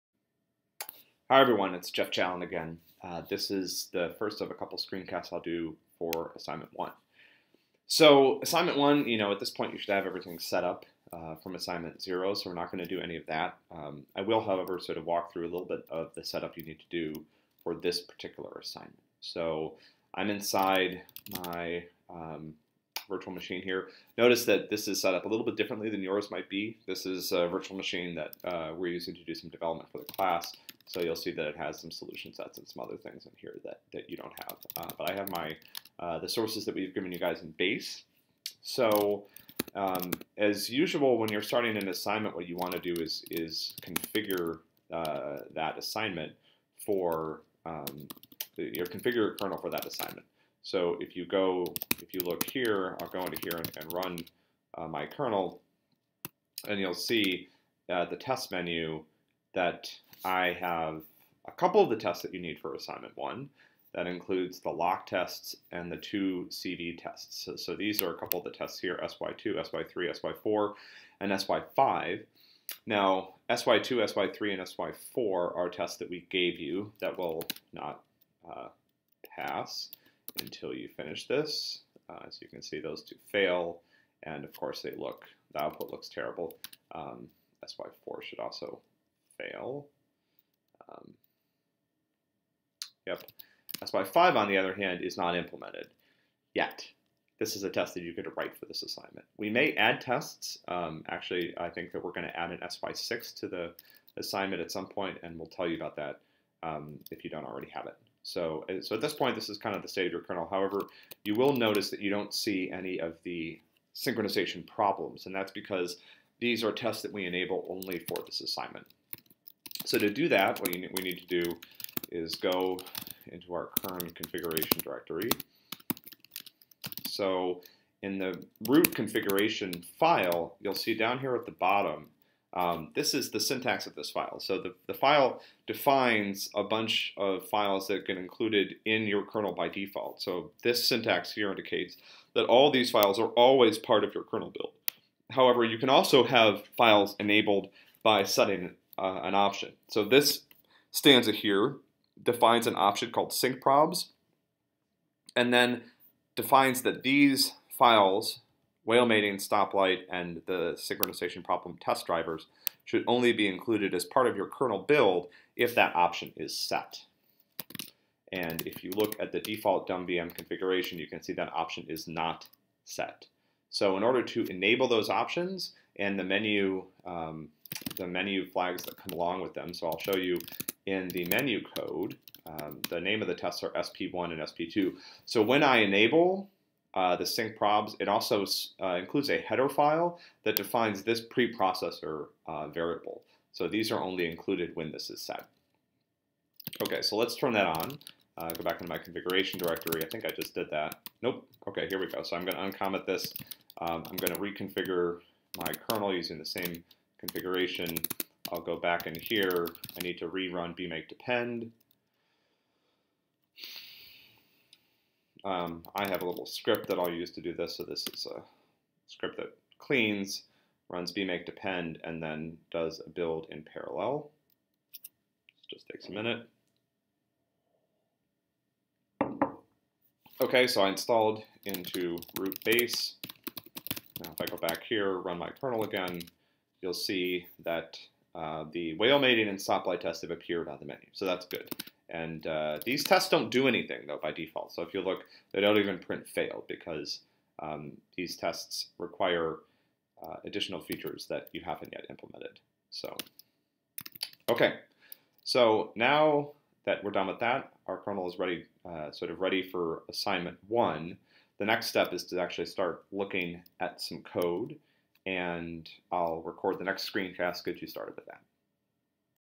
ops-class.org ASST1 Part 1: Setting Up for ASST1
Walks through how to configure your kernel for ASST1.